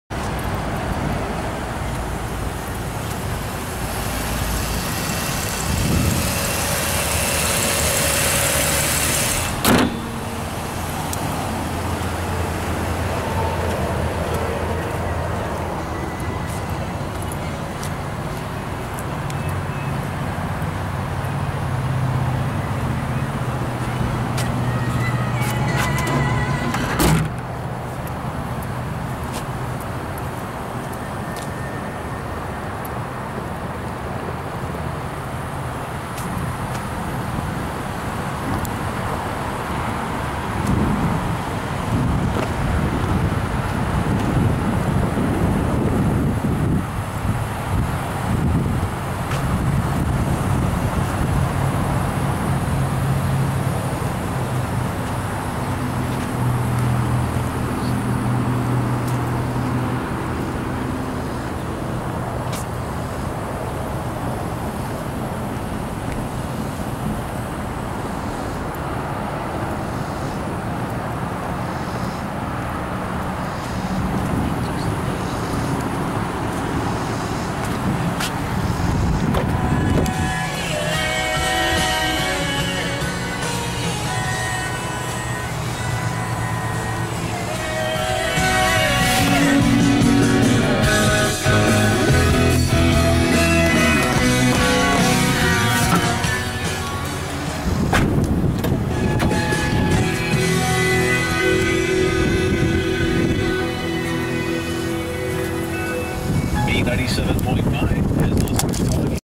FOR SALE 2006 Ford Taurus SE STK# 40052A
At Lance Cunningham Ford, YOU'RE #1! Hurry and take advantage now! HERE'S THE DEAL!! When was the last time you smiled as you turned the ignition key? Feel it again with this superb-looking 2006 Ford Taurus.